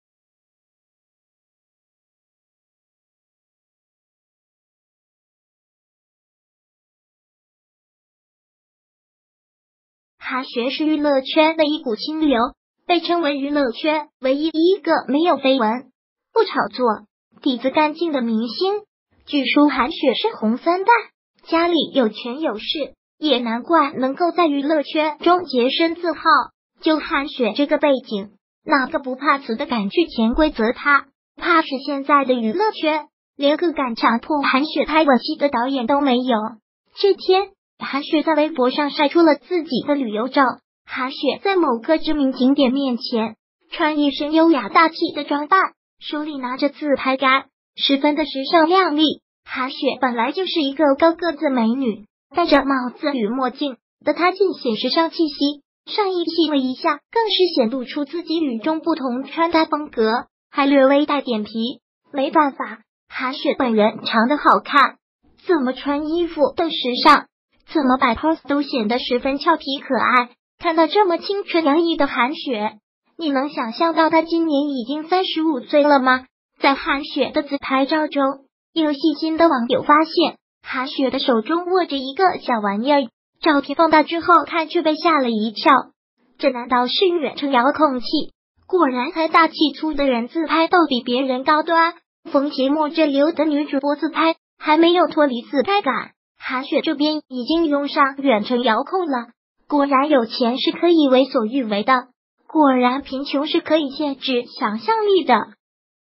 韩雪晒旅游自拍，手中的“小玩意”暴露身家背景！
韩雪是娱乐圈的一股清流，被称为娱乐圈唯一一个没有绯闻，不炒作，底子干净的明星！据说韩雪是红三代，家里有权有势，也难怪能够在娱乐圈中洁身自好。就韩雪这个背景，哪个不怕死的敢去潜规则她？怕是现在的娱乐圈，连个敢强迫韩雪拍吻戏的导演都没有。这天，韩雪在微博上晒出了自己的旅游照，韩雪在某个知名景点面前，穿一身优雅大气的装扮，手里拿着自拍杆，十分的时尚靓丽！韩雪本来就是一个高个子美女，带着帽子与墨镜的她尽显时尚气息，上衣系了一下更是显露出自己与众不同的穿搭风格，还略微带点皮。没办法，韩雪本人长得好看，怎么穿衣服都时尚，怎么摆poss都显得十分俏皮可爱！看到这么青春洋溢的韩雪，你能想象到她今年已经35岁了吗？在韩雪的自拍照中，有细心的网友发现，韩雪的手中握着一个小玩意儿，照片放大之后看却被吓了一跳！这难道是远程遥控器？ 果然财大气粗的人自拍逗比别人高端，冯提莫之流的女主播自拍还没有脱离自拍杆，韩雪这边已经用上远程遥控了！ 果然有钱是可以为所欲为的，果然贫穷是可以限制想象力的！